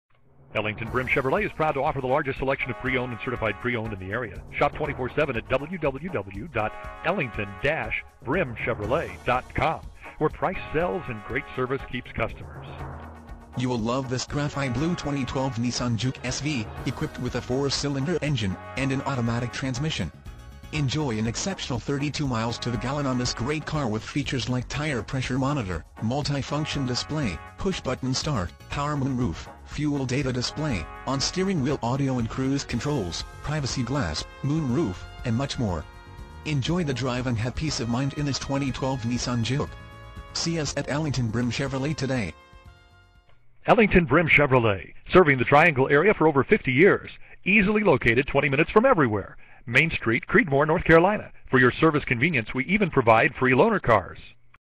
Nissan JUKE, Ellington-Brim Chevrolet- Creedmoor, NC 27522
Nissan JUKE, Ellington-Brim Chevrolet check out this Graphite Blue 2012 Nissan JUKE, equipped with a 4 Cyl. engine and an automatic transmission with 17,008 miles. enjoy an exceptional 31 miles to the gallon on this great car with features like Tire Pressure Monitoring System, multifunction display, push button start, power one-touch tilt open and close sunroof, fuel data display, on steering wheel audio and cruise controls, privacy glass, glass, and much more. Enjoy the drive and have peace of mind in this 2012 Nissan JUKE. see us at Ellington-Brim Chevrolet today!

miles- 17,008

vin- JN8AF5MRXCT102678

Ellington-Brim Chevrolet

107 Lyon ST

Creedmoor, North Carolina 27522